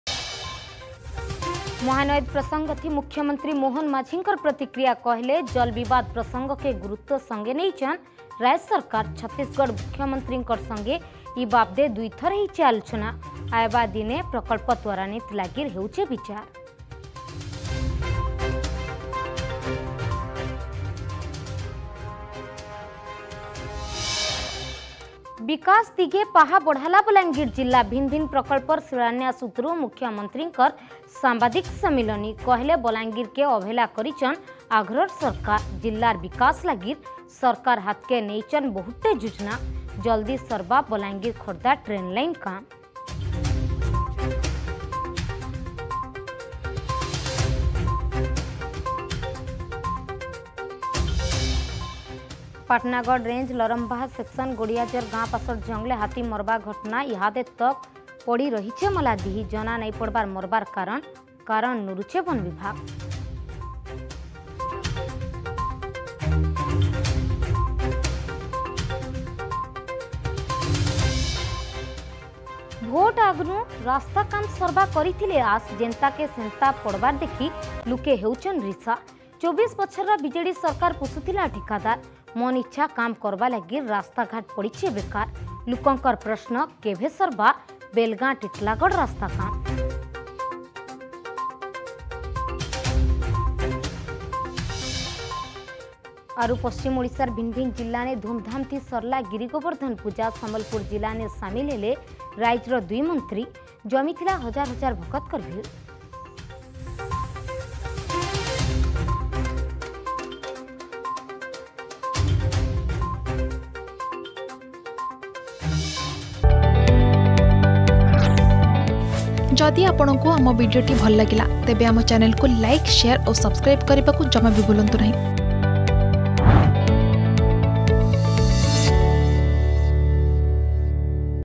Headlines 4PM | 3 Nov 2024 | ArgusNews
+ 'ସମାଧାନ ପାଇଁ ଆଲୋଚନା ଚାଲିଛି’
+ ବିକାଶମୁଖୀ ବଲାଙ୍ଗିର
+ ହାତୀ ମରବାର୍ କାରନ୍ କାଣା ?
+  କେଭେ ସରଭା ରାସ୍ତା କାମ୍ ?